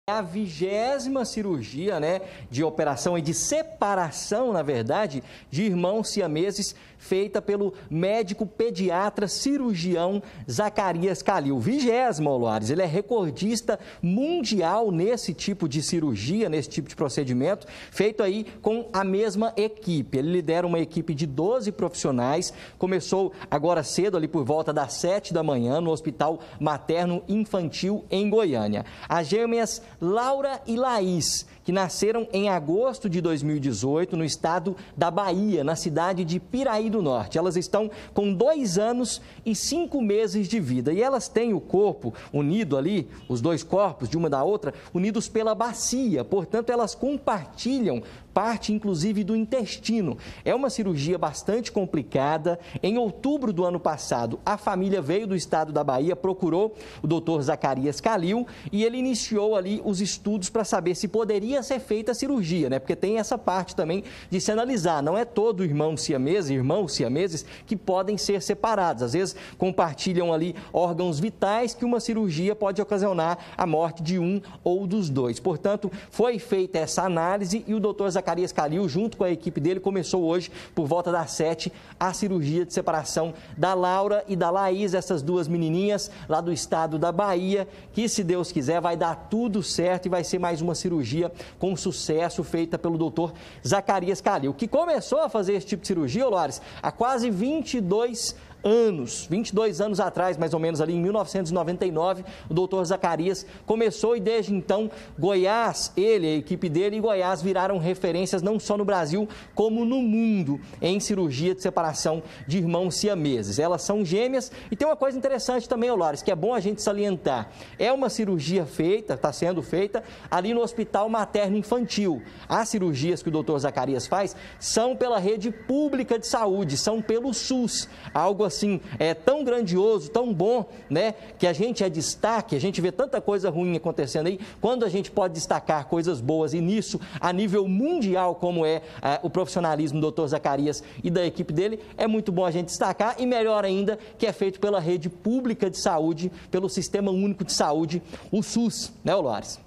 HOSPITAL MATERNO INFANTIL: GÊMEAS SIAMESAS PASSAM POR CIRURGIA DE SEPARAÇÃO
CIRURGIA DE SEPARAÇÃO DAS GÊMEAS SIAMESES QUE VIERAM DA BAHIA.